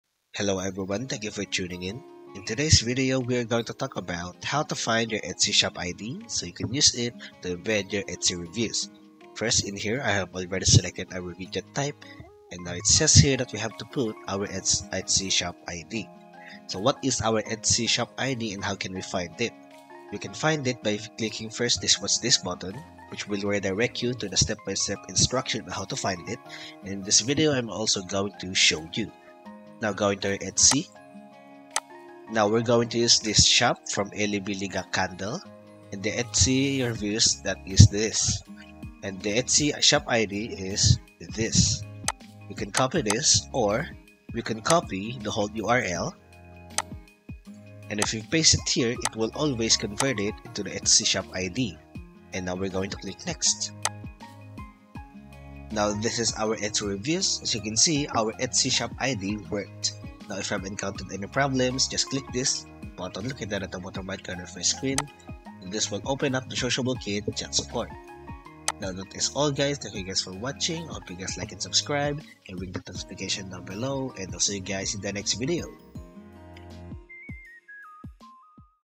How to find Etsy Shop ID? Step-by-step guide!
It might be helpful for your web or mobile application as well. This blog will show you how to find an Etsy Shop ID, step-by-step!

Why find your Etsy Shop ID?

There can be different reasons why you need to find an Etsy Shop ID. In some cases, you need the Etsy Shop ID because you want toembed Etsy reviews on your website.